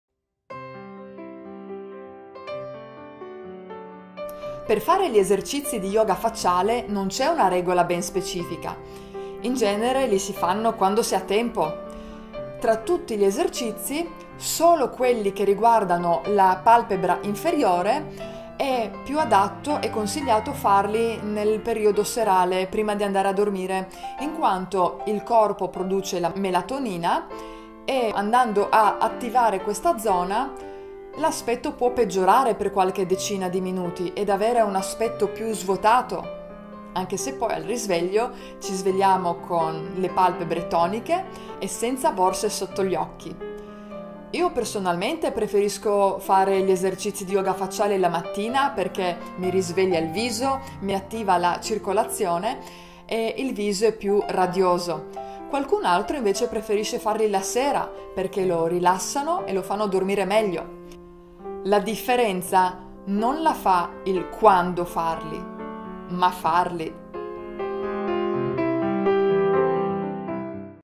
YOGA FACCIALE: qual'è l'orario ideale per allenare il viso?
Lo yoga facciale si pratica principalmente quando si ha tempo. Solo gli esercizi per la palpebra inferiore è più consigliato farli prima di andare a dormire.

La DIFFERENZA comunque non la fa il QUANDO FARE gli esercizi, ma FARLI!!